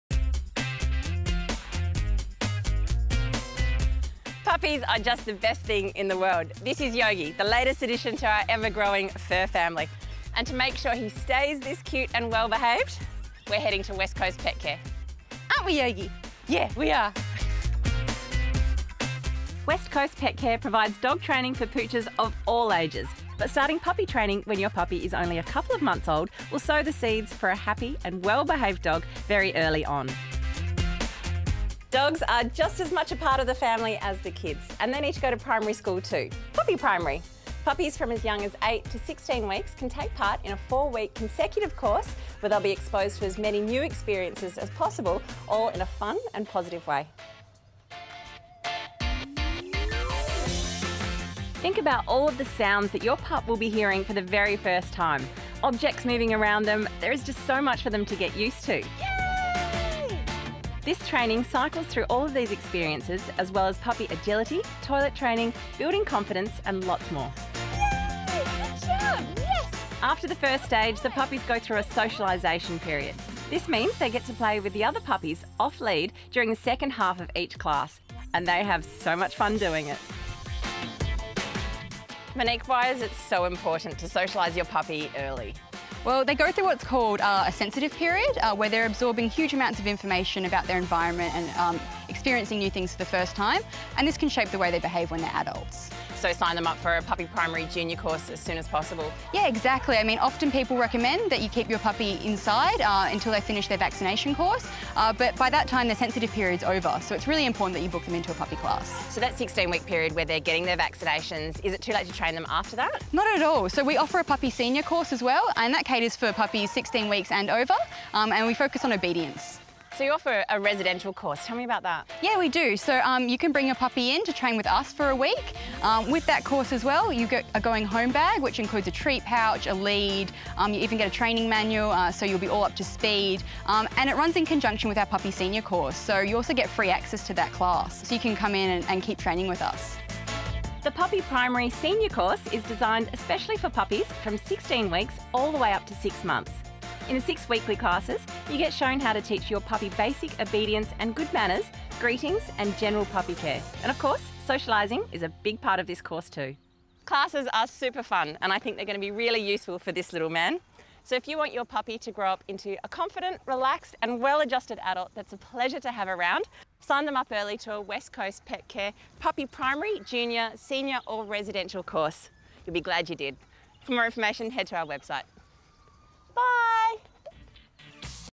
Puppy Training - West Coast Pet Care Centre
Starting puppy training when your new bundle of joy is only a couple of months old will sow the seeds for a obedient and well mannered dog in addition to a great bond between you and the dog. We have a selection of puppy training options that will get your new family member off to the perfect start.